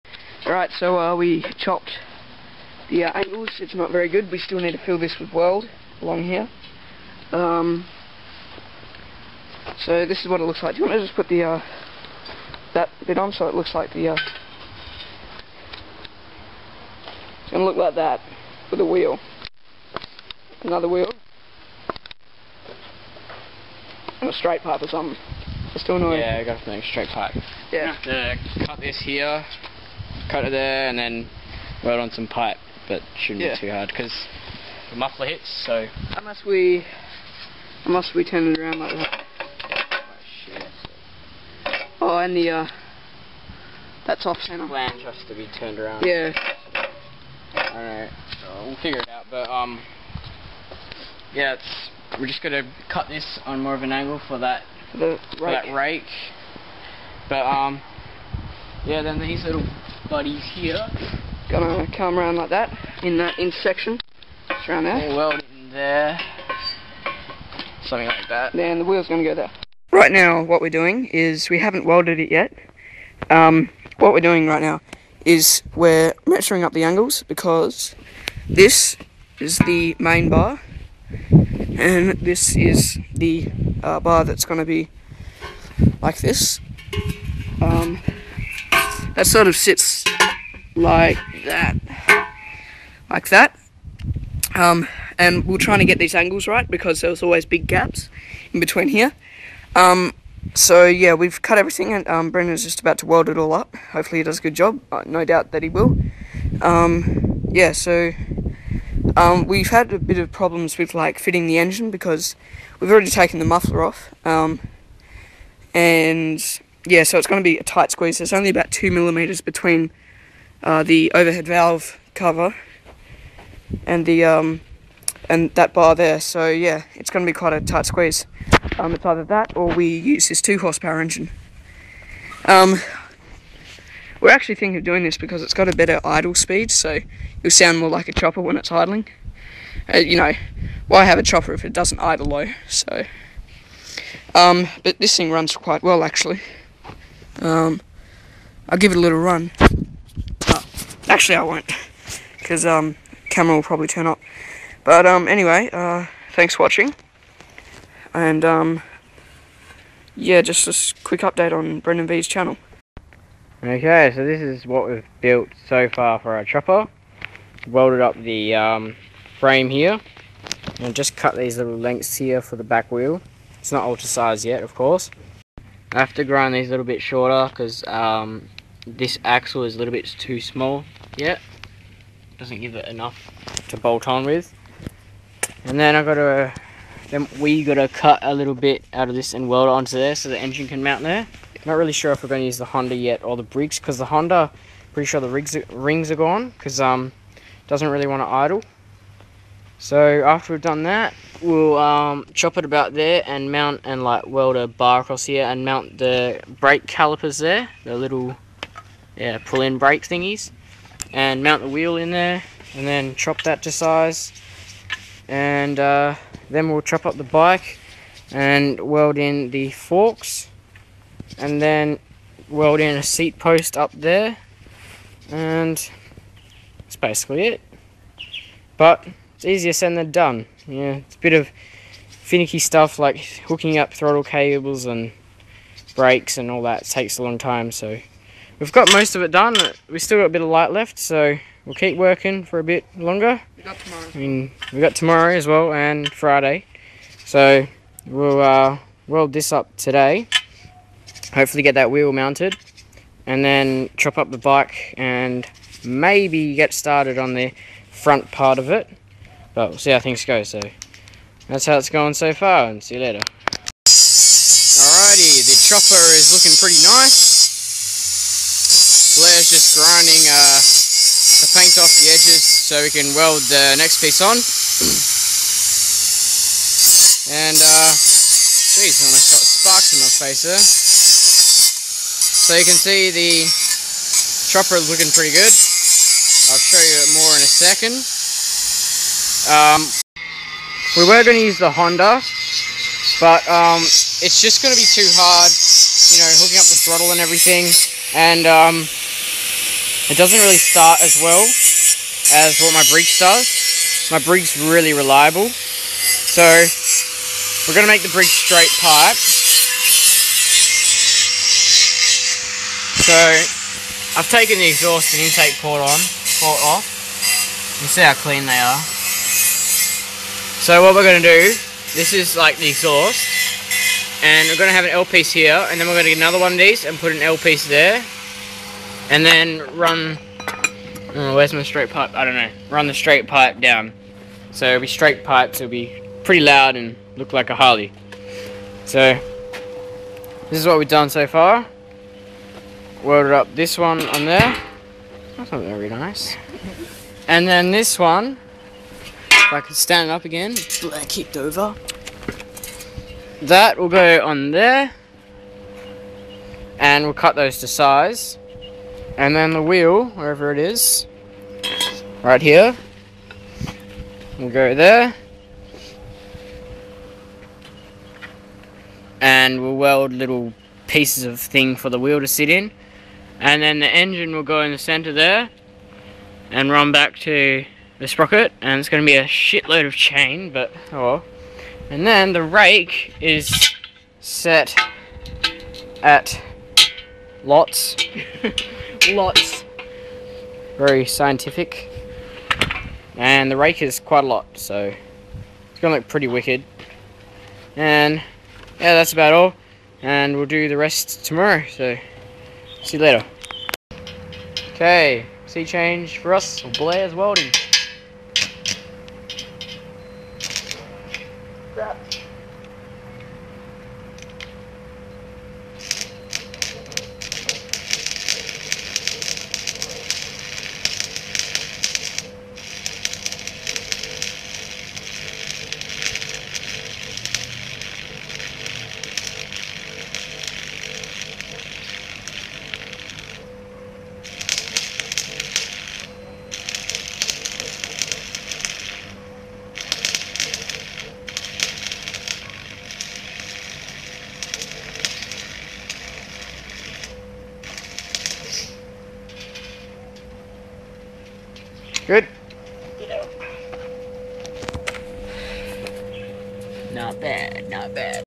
Mini Chopper Build Part 1 made by Brendonv101 and Cchange4us
Time Restriction: 3 days
Objective: Make a ride able mini chopper with little or no money spent.

The chopper frame was built out of exercise equipment that was collected from the rubbish collection. The forks, seat, wheel holders and a few other parts like cables and levers were taken from a scrap mountain bike also found in the rubbish collection. The engine came off an edger which was donated by a friend of my dads. The chain was given to us by my dads friend. He has a roll of about 15m of 35 pitch chain so he said take what ever you want. The wheels came from a small kids bike and the sprocket came from ebay. All metal used came from the scrap.

The total cost is:
Sprocket: $36.00
Exhaust system and motor mounting hardware: $14.00
Chain idlers: $6.00 (yet to buy these but i will)

Total: $56.00 AUD